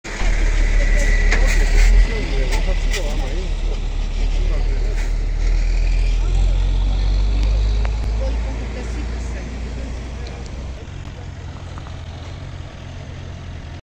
kozan.gr: Τροχαίο ατύχημα με εμπλοκή τριών αυτοκινήτων
Τροχαίο ατύχημα με εμπλοκή τριών αυτοκινήτων σημειώθηκε το μεσημέρι της Δευτέρας 8/4, λίγο μετά τη 1:25, στη διασταύρωση των οδών Κολοκοτρώνη με Ιωνίας στην Κοζάνη. Eυτυχώς σημειώθηκαν μόνο υλικές ζημιές.